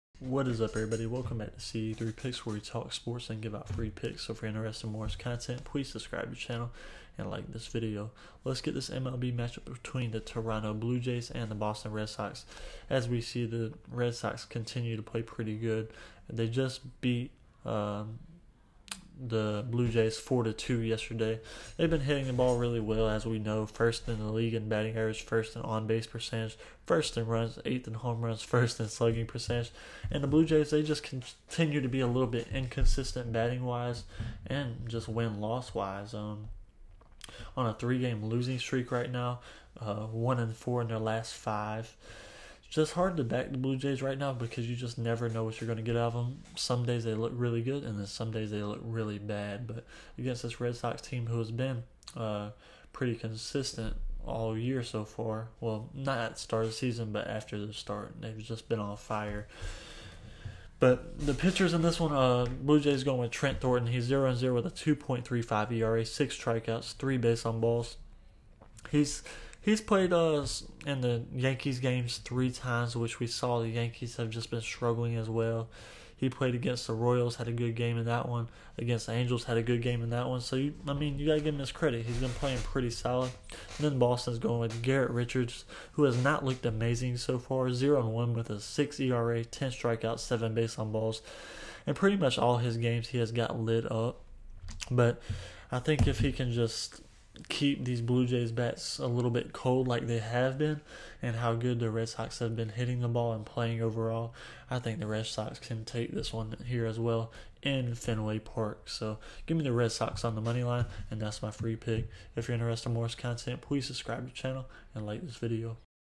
Blue Jays vs Red Sox 4/21/21 Free MLB pick, tip, and projection.
The Toronto Blue Jays take on the Boston Red Sox on April 21st, 2021.

Instagram.com/ceiiipicks
Twitter.com/ceiiipicks